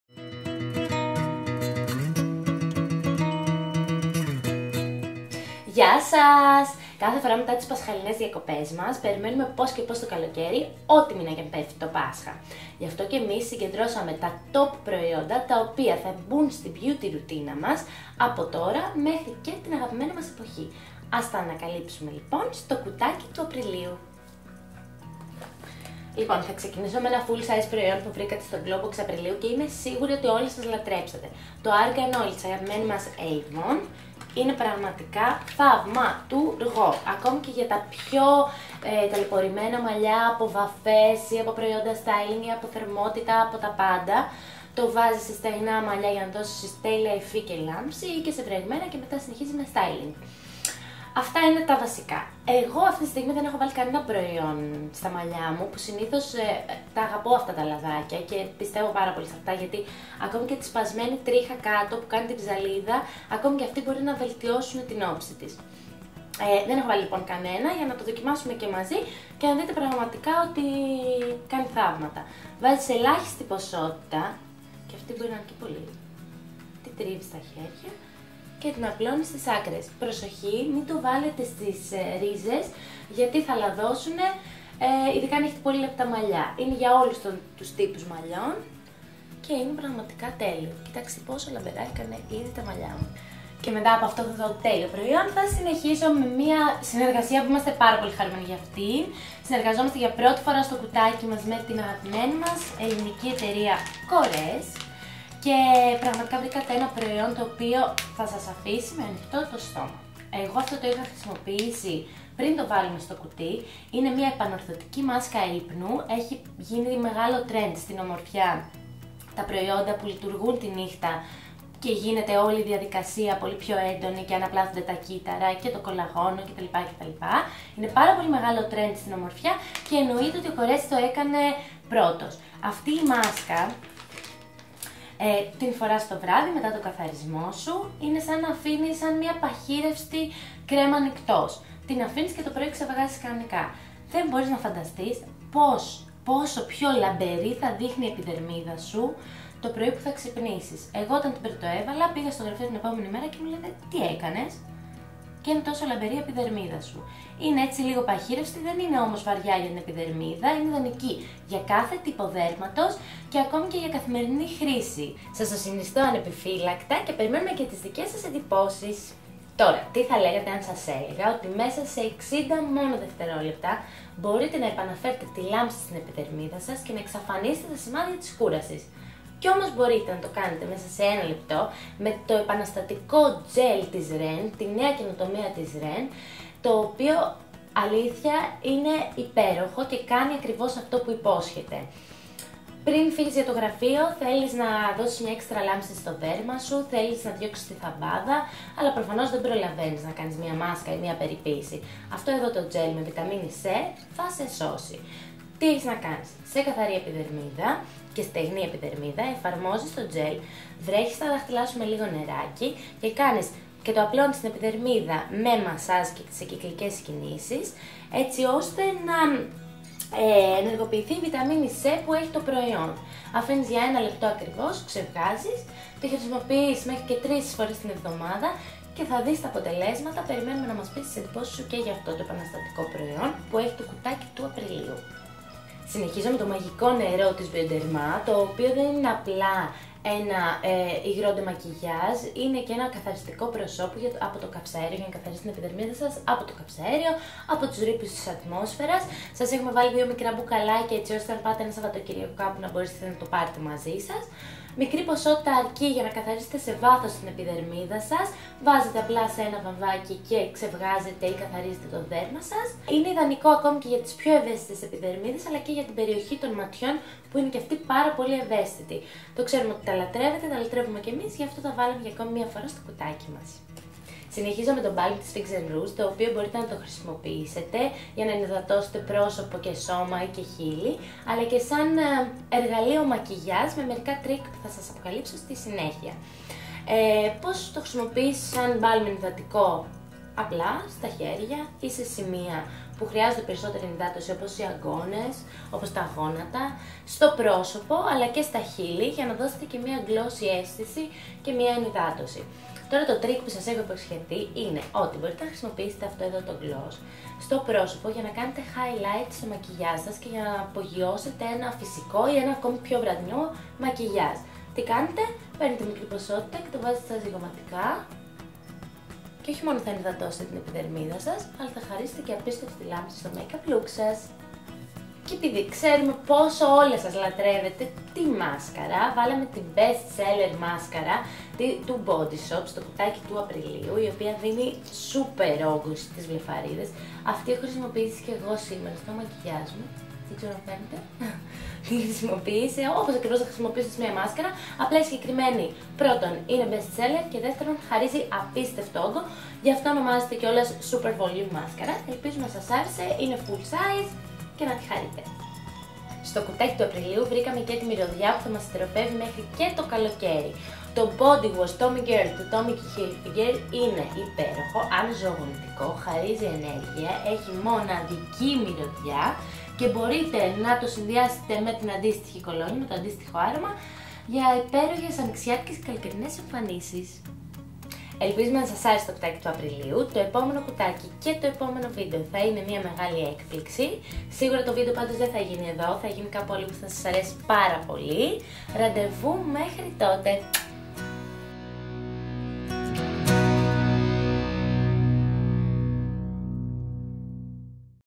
April Glowbox 2015
Το κουτί περιέχει μια επιλογή ( 5 έως 7 ) από τα παρακάτω προϊόντα:

-Αdvance Techniques Moroccan Argan Oil, Avon:
Eντατική ενυδάτωση
Επαναφορά λάμψης
Αποκατάσταση φθορών και ψαλίδας

-Επανορθωτική μάσκα ύπνου Άγριο Τριαντάφυλλο, Korres:
Αναπλαστική μάσκα
Επανορθώνει την επιδερμίδα
Καταπολεμά τις δυσχρωμίες

-Flash Rinse 1 minute facial, Ren:
Επαναφέρει τη λάμψη σε 1 λεπτό
Εξαφανίζει σημάδια κούρασης
3 φορές την εβδομάδα

-Sensibio H2O Solution Micellaire, Bioderma:
Μαγικό νερό ντεμακιγιάζ και καθαρισμού
Ιδανικό και για ευαίσθητες επιδερμίδες
Και για μάτια

Ενυδατικό Balm
Για πρόσωπο, σώμα και χείλη
Tip: Για έξτρα λάμψη στα ζυγωματικά

Mαύρη μάσκαρα για έξτρα όγκο
Βest Seller προϊόν

-Τοmmy Girl Body Wash, Tommy Hilfinger:
Δροσερή και φρέσκια μυρωδιά
Χαρίζει ενέργεια
Συνδύασε το με το αντίστοιχο άρωμα